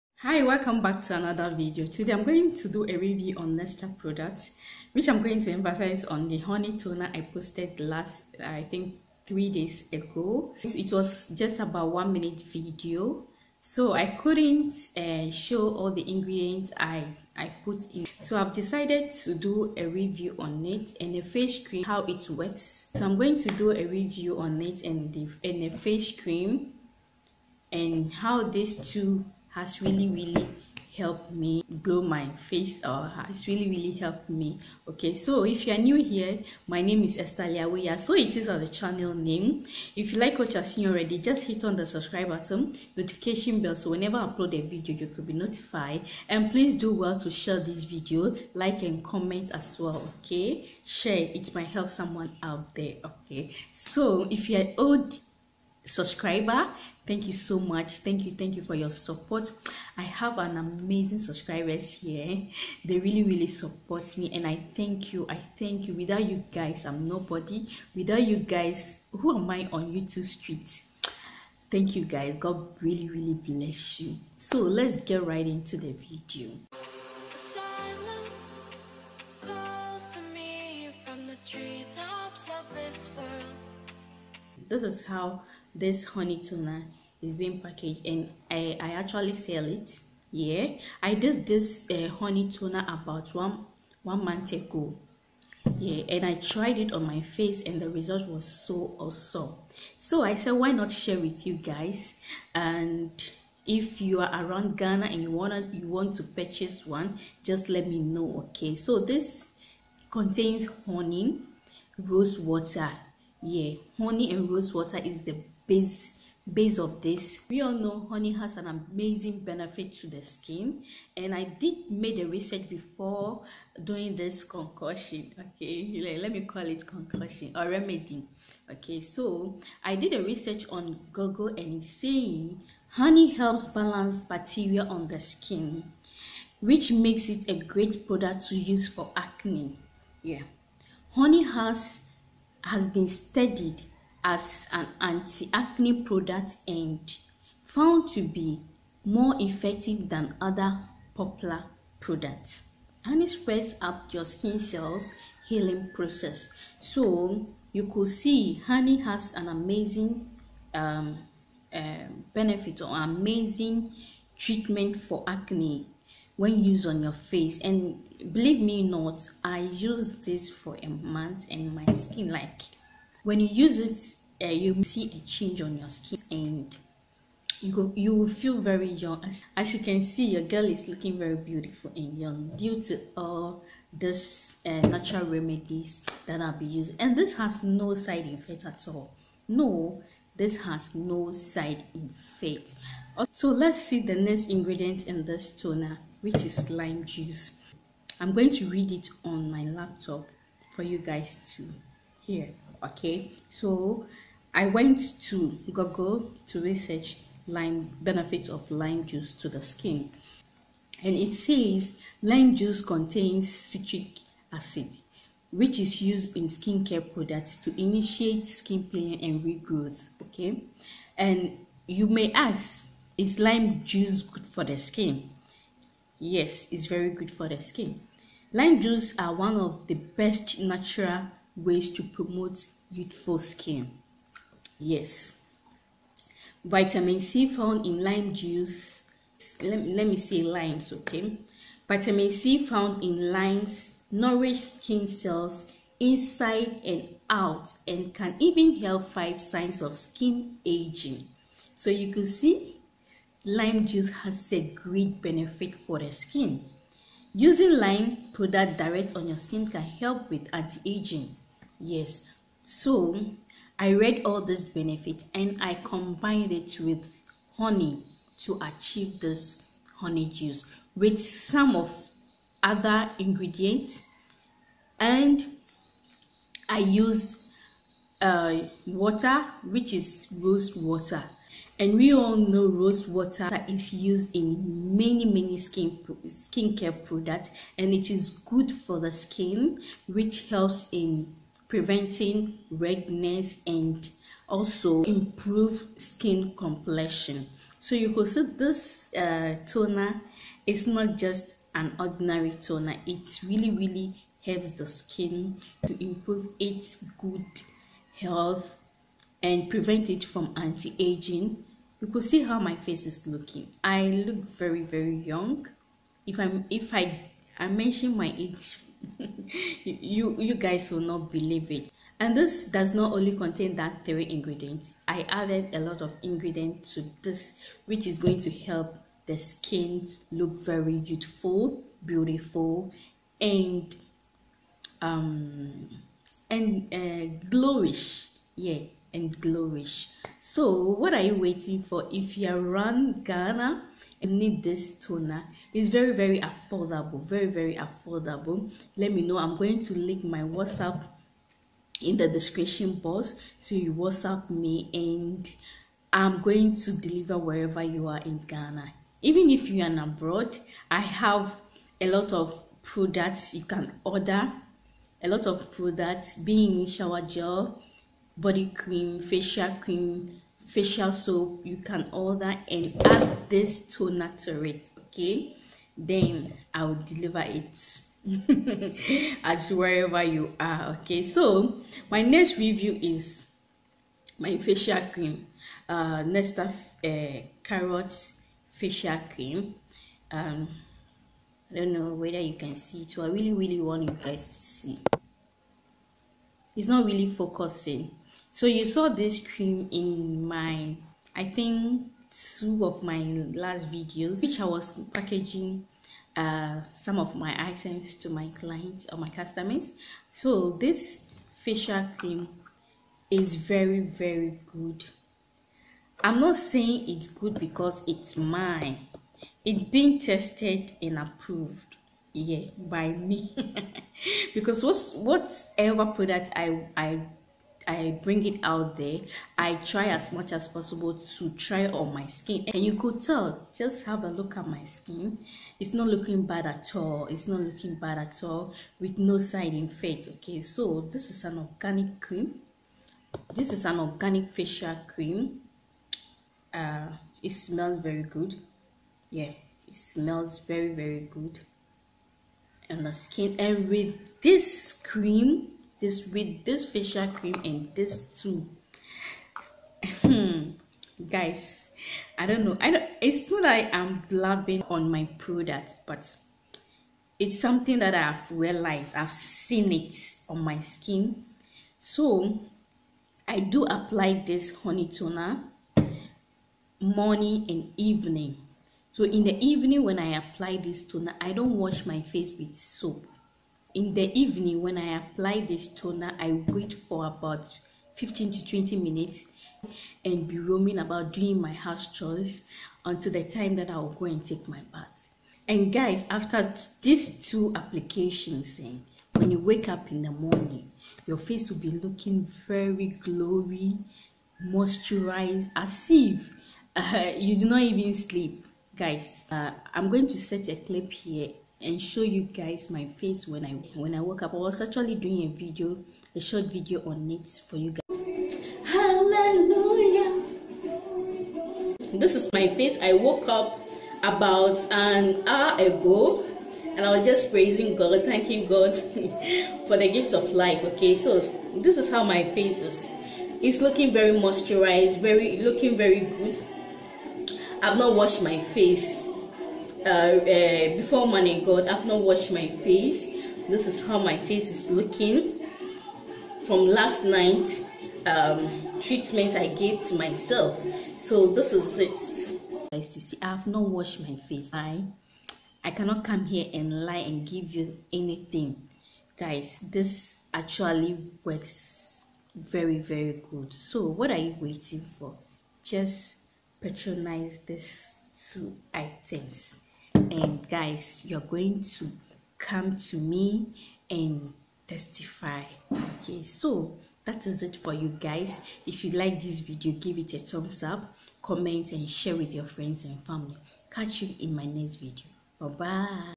Natural Honey Facial Toner & Carrot Face Cream Review//My Honest opinion Nesta honey & Cream Facial
Natural Skincare remedies are on the skin without any side effects and I love using natural ingredients in making my skin care products.

So now let's see what honey can do to skin. Honey is one of nature's most revered skin remedies. It's antibacterial and antiseptic abilities, may benefit oily and acne-prone skin. Honey is also a natural humectant, so it helps keep the skin moist but not oily. This is because humectants draw moisture from the skin without replacing it. So this is combined with lots ingredients which good for skin to formulate this honey toner.

And Nesta face cream is also a great mixture formulate treat the skin form acne, pigmentation and all other skin problems leave the skin feels better, moisturize and good looking.

So today am sharing Natural Honey Facial Toner & Carrot Face Cream Review//My Honest opinion Nesta honey & Cream Facial hope you find this very helpful.

PRODUCT MENTIONED
Honey Facial Toner
Nesta Face Cream

How To Make Honey Facial Toner / Homemade Honey Toner

Thanks to you all for your support
MUCH LOVE...